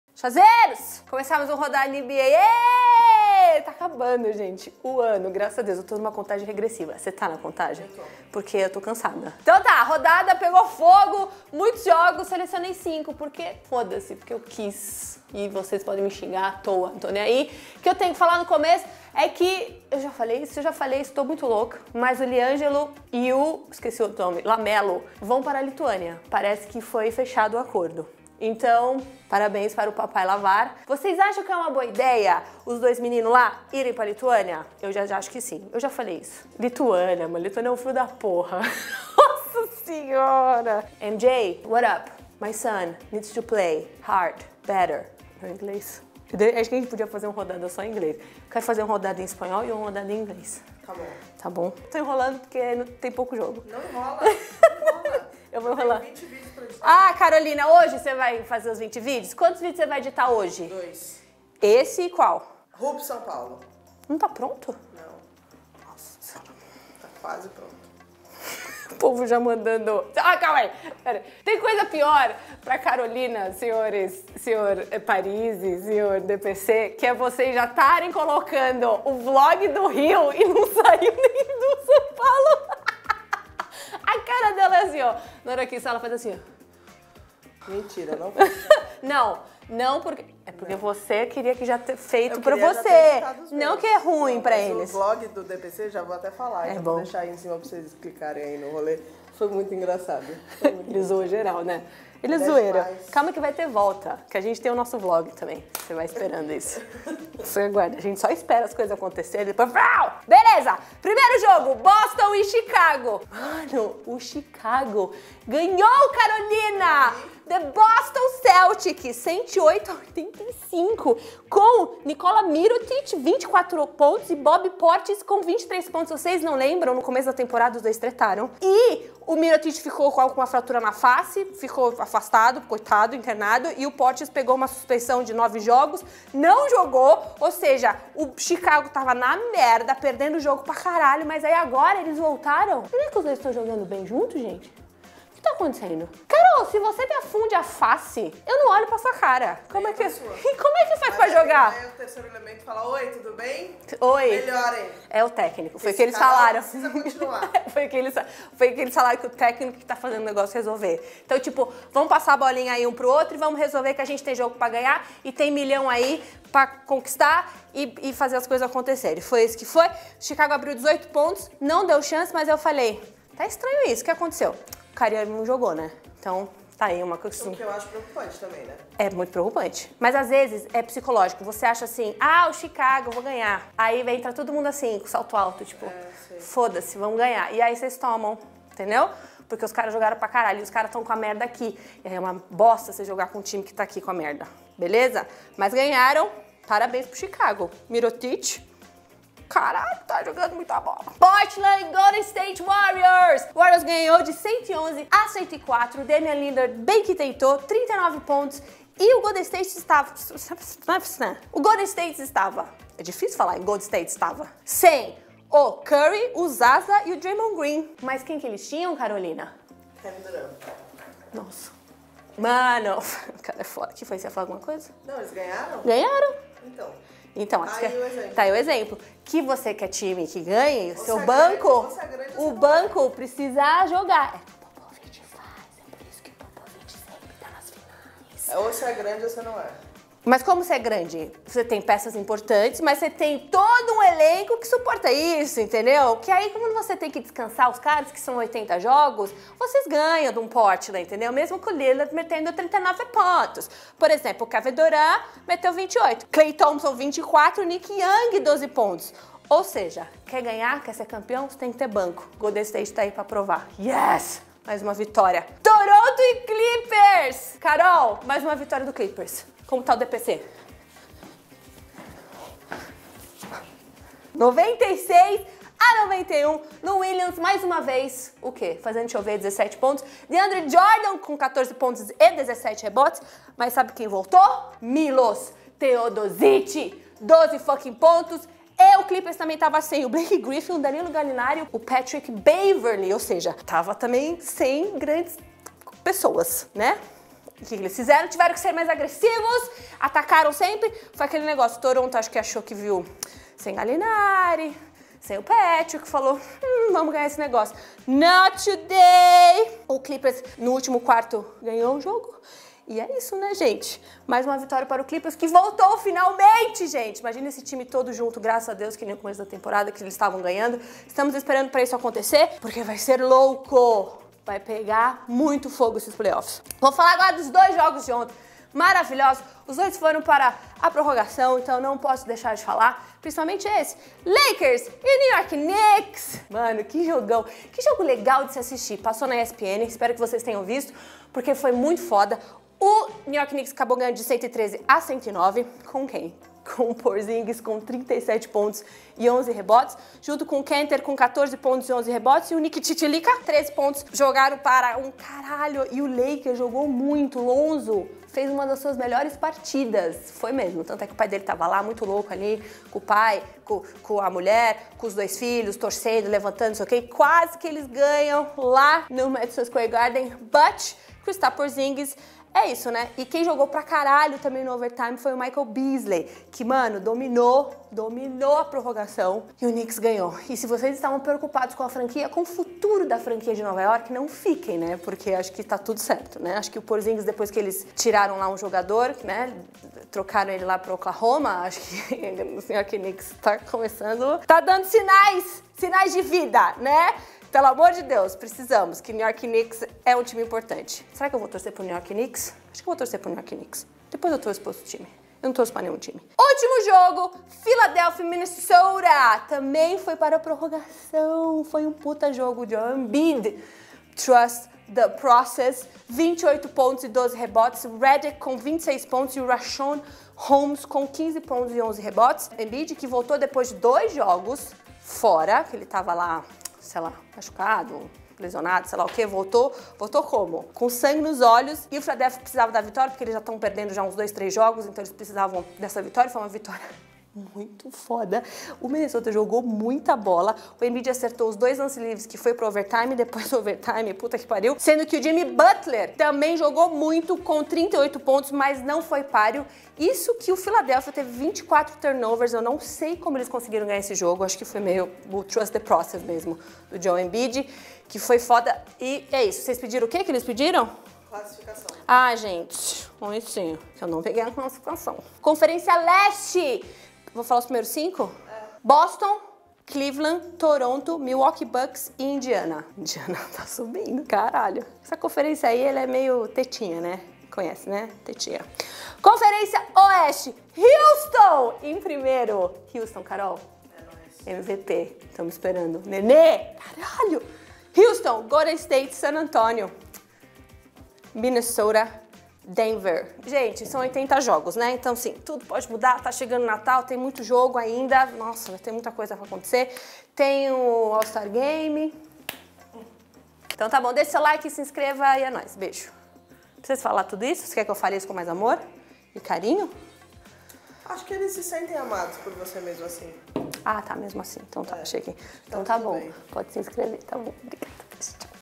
ACABOU A TRETA NO CHICAGO BULLS? - Rodada NBA 17/18 #19
Rodada dos Over Times e dos placares surpreendentes. Parece que eu estou sobre influência de tóxicos mas NÃO. ESTÁ TUDO BEM.

Produção: Mafia Produtora
Direção: Carolina Loureiro
Edição: Carolina Loureiro
Câmera: Carolina Loureiro
Roteiro: Gabriela Neves
Apresentador(es): Gabriela Neves / Bruno Marques

canalchua